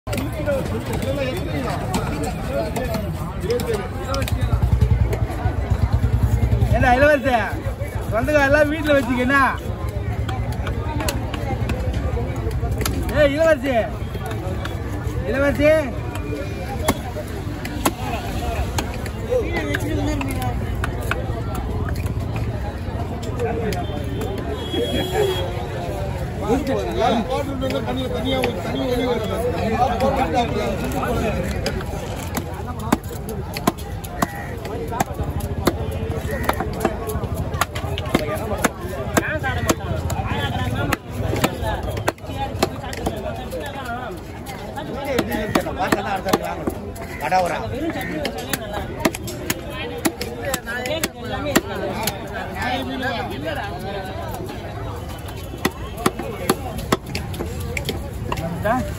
Giant Cobia Fish Cutting Skills in Kasimedu Fish Market
Here we have Showcased big Cobia/ Kadal viral Fish Cutting& Chopping in Kasimedu Fish Market, Chennai.